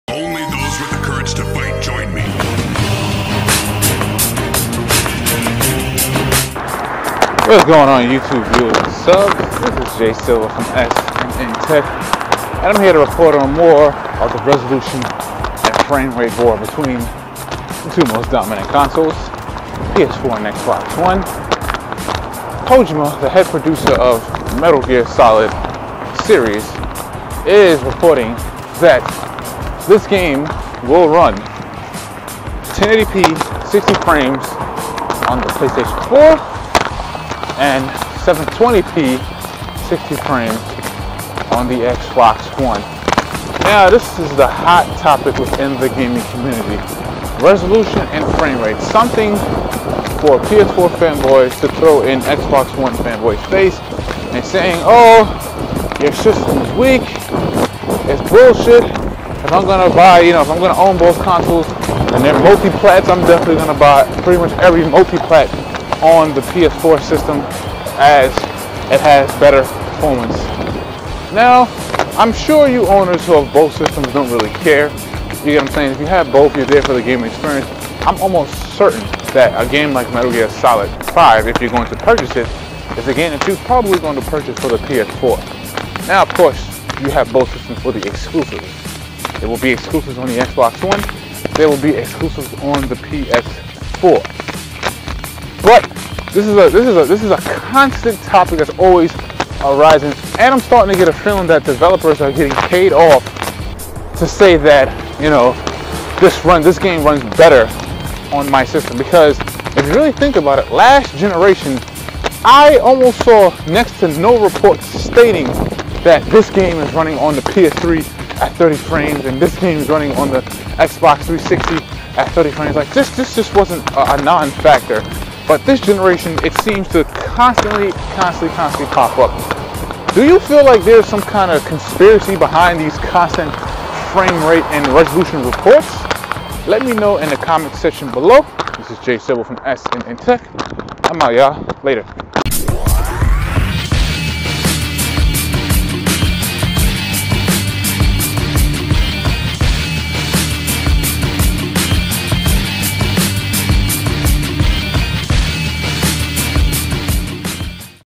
Metal gear Solid 5 1080p 60fps on the ps4 but 720p on xbox
We have yet another report of a multi-plat game that is reported to have 1080p 60fps on  the ps4 and 720p 60fps on the xbox one. yes facts is facts, but it's crazy to me how these types of reports are coming out the press left and right, knowing full well this is only gonna cause negative feedback from gamers amongst each other. Is this kind of reporting trend really necessary?

If you like my video please *thumbs up* 8)

PSN: MallyMall88_
Xbox GT: MallyMall88